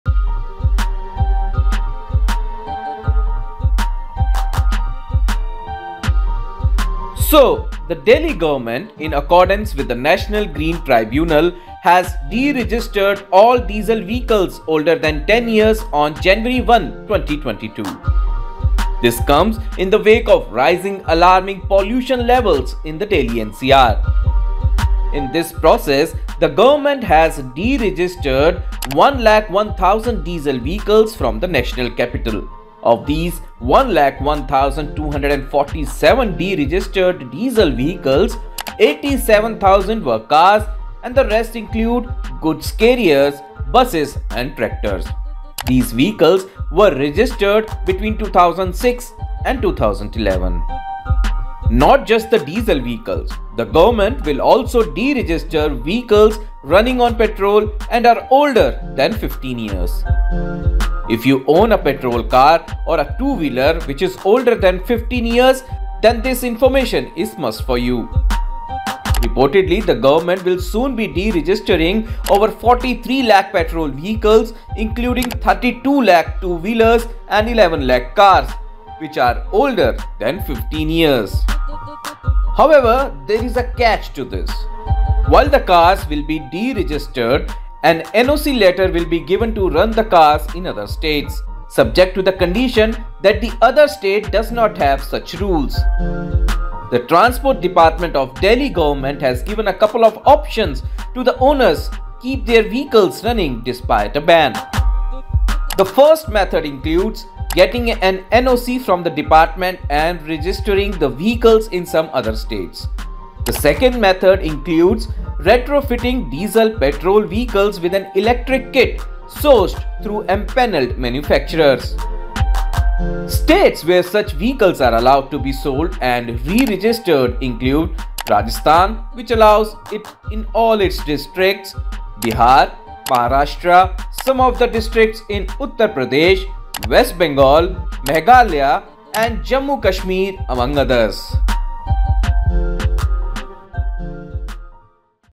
Ban on 10-year-old diesel car. What to do know? Here are few options for owners
Delhi government deregistered 10-year-old diesel cars. Owners of 10-year-old diesel vehicles have now the option to retrofit them with electric kits or sell them in other states.